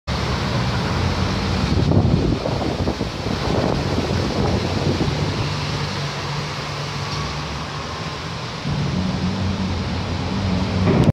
Relampago en Buenos Aires . 21/02/2017
Un relampago de los cientos durante las horas que duro la tormenta. Algunos cubrian la mitad del cielo.